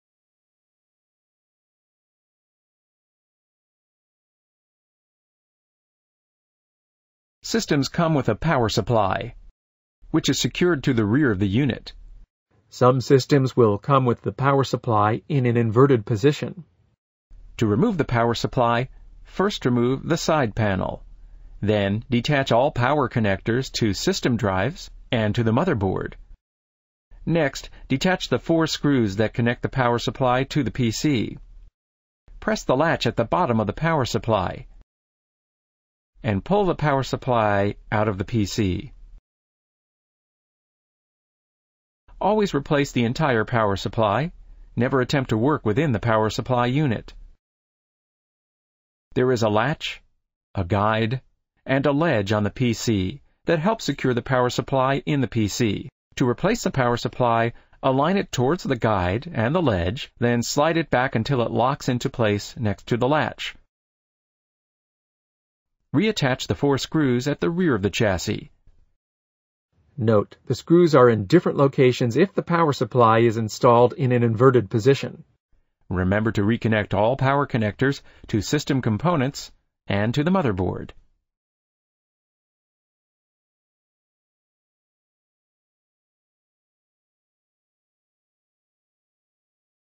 Replace Power Supply | Presario CQ5000 | HP Support
Learn how to remove and replace the power supply for the Compaq Presario CQ5000 series desktop.